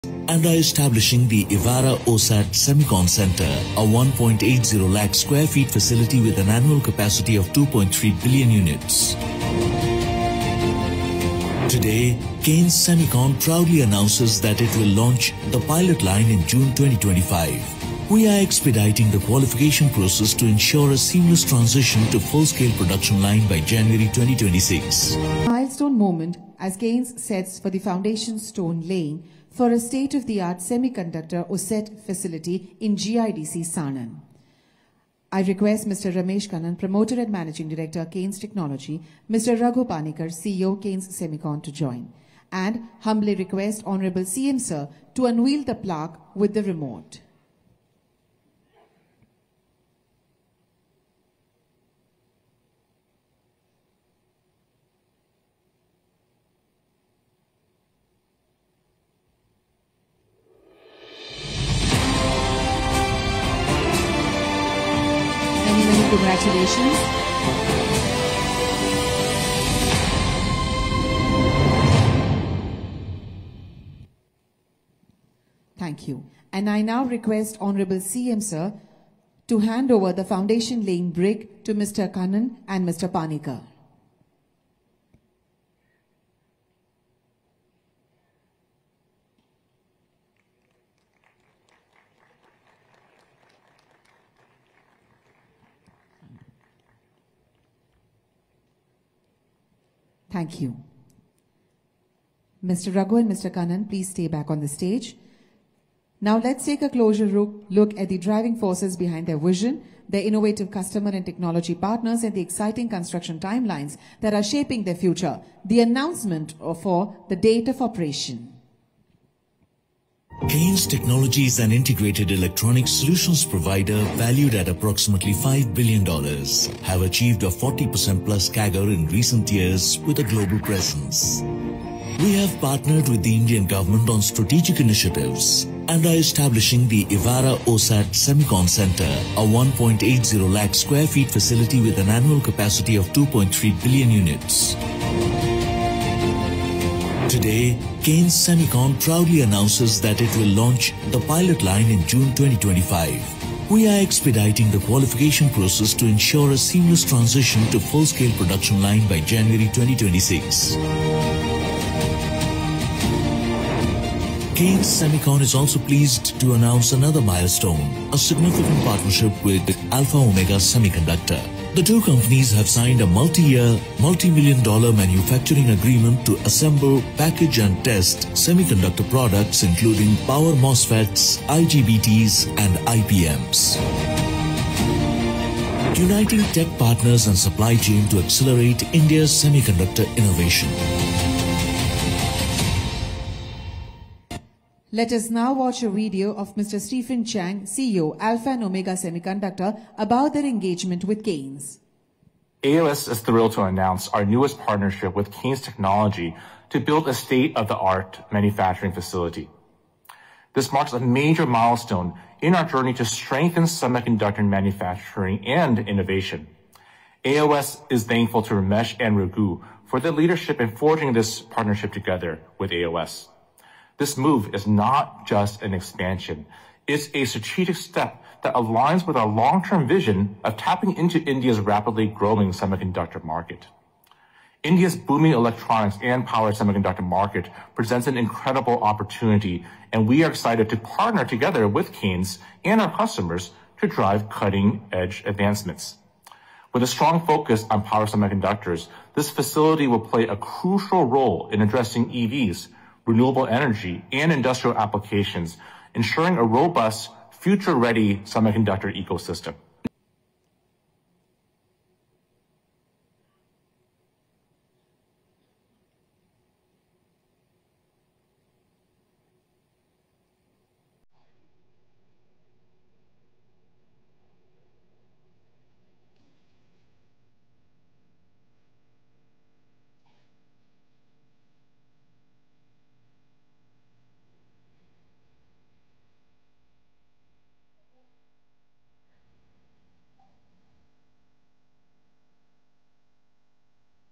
Sanand to get Kaynes OSAT Evara Semiconductor plant
Gandhinagar: Sanand in Gujarat is set to get another Semiconductor unit. The foundation stone-laying ceremony for Kaynes Semicon OSAT Evara Semicon Centre was performed today virtually by Chief Minister Bhupendra Patel at the Gujarat Seconnect conference in Mahatma Mandir here. The plant will come up at the GIDC area in Sanand, where Micron's semiconductor plant is also being developed.

On this occasion, the company announced that the production of semiconductor chips is going to start in June 2025 through the pilot manufacturing line at this semiconductor OSAT plant of Kaynes in Sanand and in January 2026 through the main manufacturing line. Kaynes announced that it is establishing a 1.80 lakh square ft facility at Sanand GIDC with an annual capacity of 2.3 billion units. Pilot line will be up in June 2025, and the plant one will be ready in January 2026.

Kaynes, a technology and integrated electronics solutions provider (Kaynes circuit PCB plus EMS and Kaynes Semicon OSAT), is valued at approximately five billion dollars. It has achieved over 40 percent CAGR growth in recent years with a global presence.  Kaynes Semicon partnership with APS for assembly, packaging, and testing power MOSFETs, IGBTs, and IPMs semiconductor products. Kaynes has partnered with IIT Gandhinagar, NAMTECH Sanand, IIT Bombay, DY Patil University Pune, IIT Kanpur, W Beyond Telangana, SASTRA University Thanjavur, and MACDERMID Alpha Chennai for capacity building.

Kaynes has partnered with the Indian government arms ISRO, Railways - Kawach, Aerospace and Defence, Rudra+C-DAC, and for semiconductor and OSAT expansion.

It was reported in August 2024 that Electronics manufacturing company Kaynes Technology acquired land in Sanand to establish its outsourced semiconductor assembly and test (OSAT) unit and the company plans to invest around Rs 5,000 crore in this chip project.

The Union Cabinet chaired by Prime Minister Shri Narendra Modi in September 2024 approved the proposal of Kaynes Semicon Pvt Ltd to setup a semiconductor unit in Sanand, Gujarat. It was announced that the proposed unit will be setup with an investment of Rs 3,300 crore and the capacity of this unit will be 60 Lakh chips per day.

The chips produced in this unit will cater to a wide variety of applications which include segments such as industrial, automotive, electric vehicles, consumer electronics, telecom, mobile phones, etc.

In June, 2023, the Union Cabinet had approved the first proposal for setting up a semiconductor unit in Sanand, Gujarat. In February, 2024, three more semiconductor units were approved. Tata Electronics is setting up a semiconductor fab in Dholera, Gujarat and one semiconductor unit in Morigaon, Assam. CG Power is setting up one semiconductor unit in Sanand, Gujarat

DeshGujarat